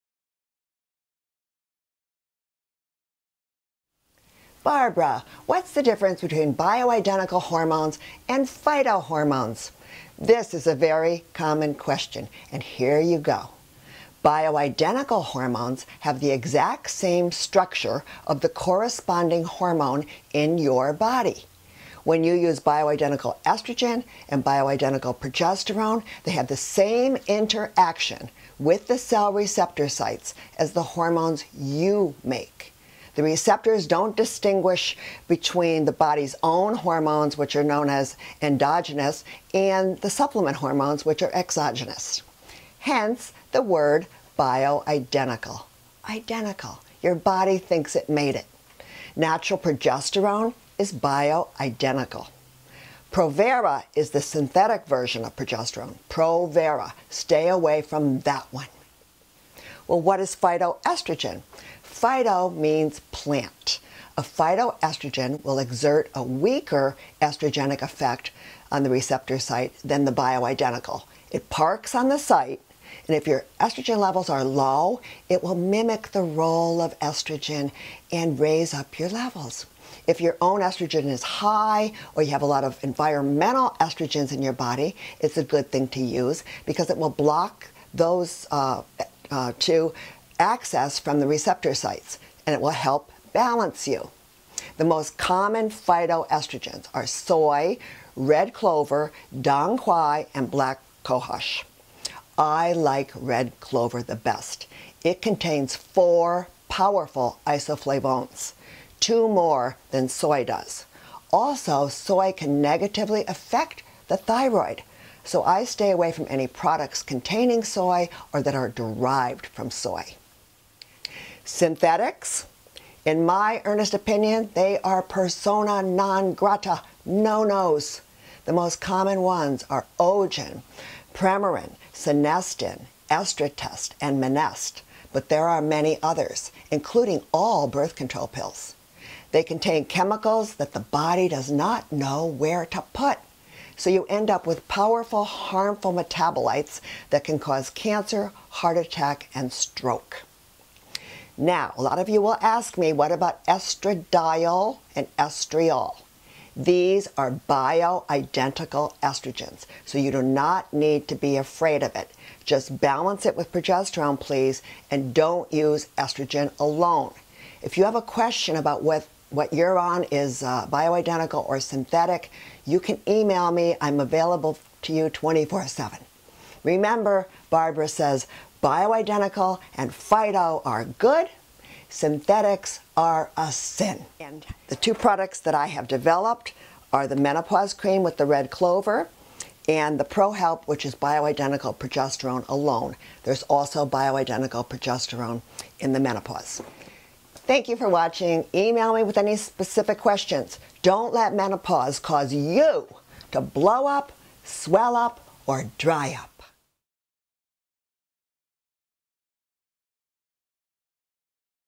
Which Estrogens Are Safe?
Barbara Hoffman explains the differences between BIO-IDENTICAL, PHYTO, and SYNTHETIC hormones. Confused about which estrogens and progesterones are safe to use? Not sure what the differences really mean? Barbara knows.